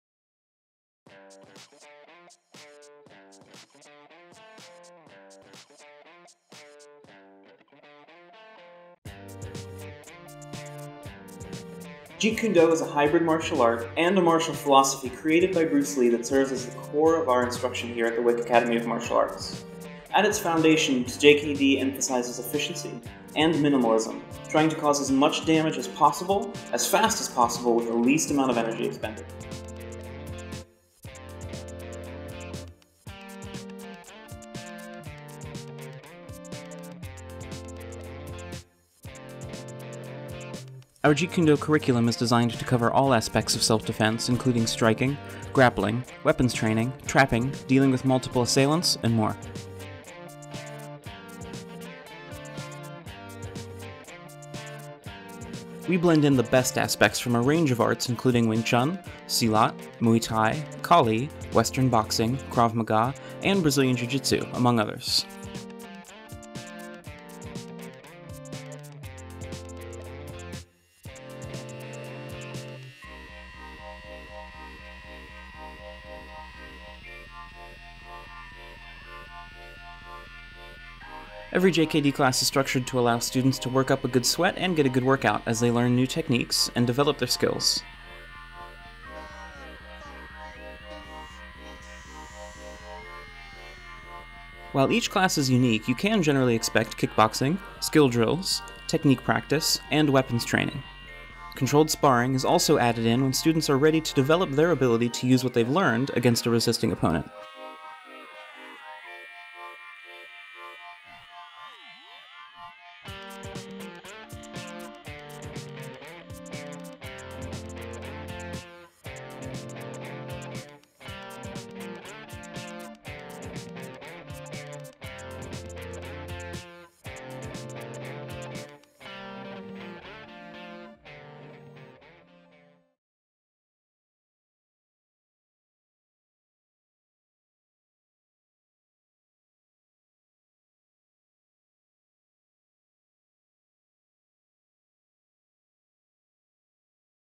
About Jeet Kune Do at The Wik Academy of Martial Arts
Jeet Kune Do forms the core of our instruction at The Wik Academy of Martial Arts. Learn a little about what JKD is, and how it's taught at the Academy.